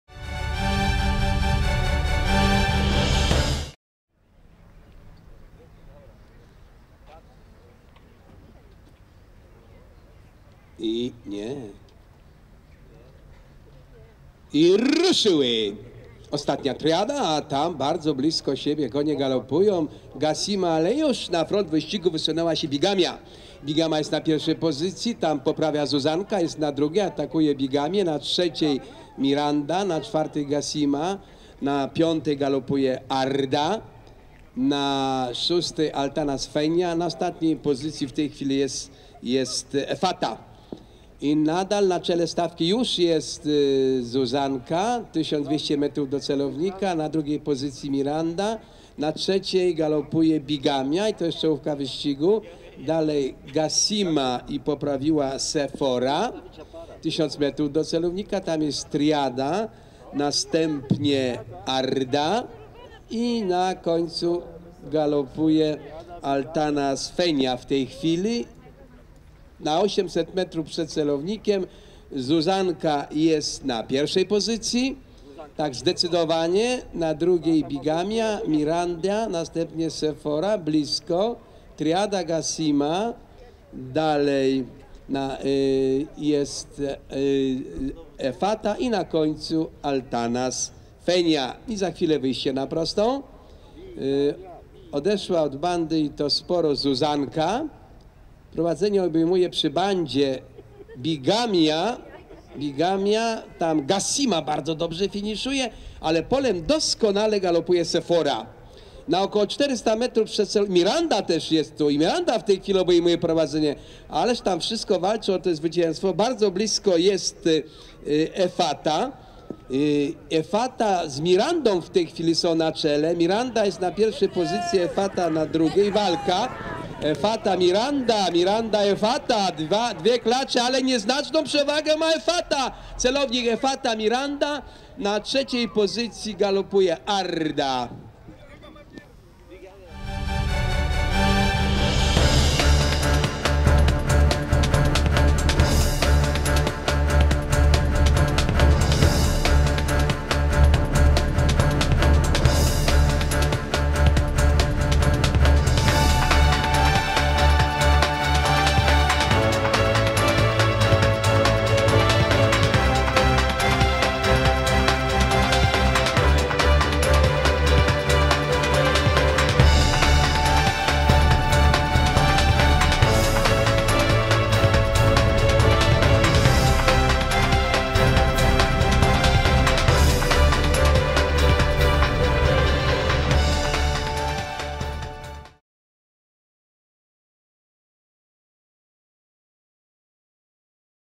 5.10.2008 (D30;G6;tor: 3,3; 13°C)
Najpierw prowadziła Bigamia, potem Zuzanka, następnie znów po wyjściu na prostą Bigamia. Ale decydujący atak nastąpił w połowie prostej. Wyrwała do przodu Efata pod Jerzym Ochockim, z którą nawiązała walkę najmocniej grana Miranda. Zacięta walka trwała do celownika. Wygrała ją o łeb Efata. Na prostej przez chwilę zaistniała też Sefora, która jednak nie obroniła trzeciej pozycji przed finiszującą Ardą. Nic dziwnego, że w tym arabskim "przekładańcu" nikt nie trafił czwórki. Trenowaną przez Tomasza Kluczyńskiego Efatę wyhodował G. Sęczek. Klacz jest własnością D. Zalewskiego.